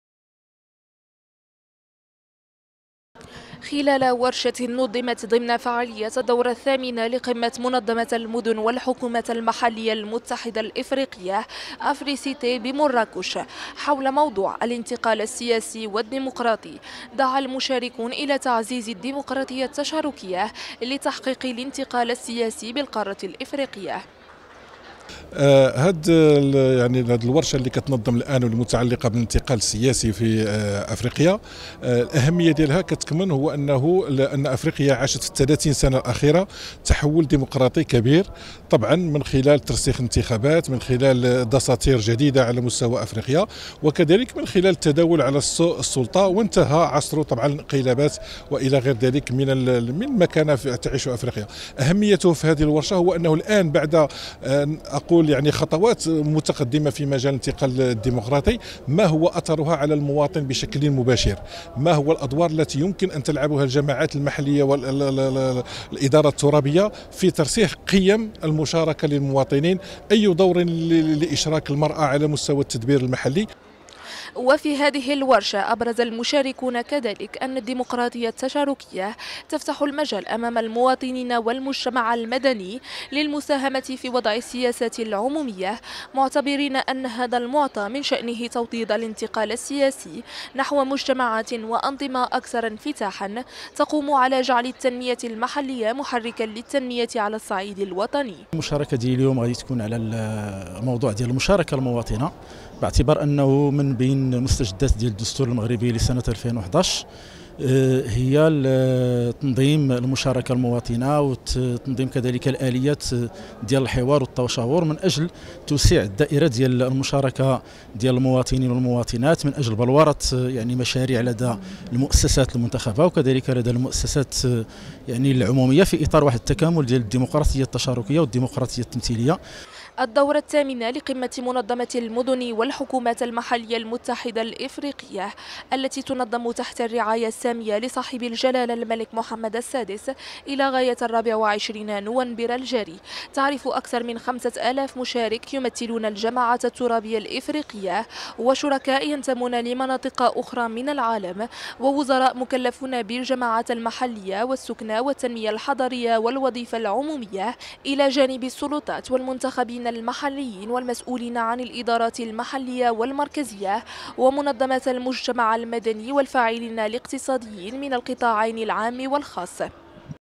أفريسيتي 2018.. الدعوة إلى تعزيز الديمقراطية التشاركة لتحقيق الانتقال السياسي بالقارة الإفريقية
خلال ورشة نظمت ضمن فعاليات الدورة الثامنة لقمة منظمة المدن والحكومات المحلية المتحدة الإفريقية "أفريسيتي" بمراكش، حول موضوع "الانتقال السياسي والديمقراطي" دعا المشاركون إلى تعزيز الديمقراطية التشاركية لتحقيق الانتقال السياسي بالقارة الإفريقية.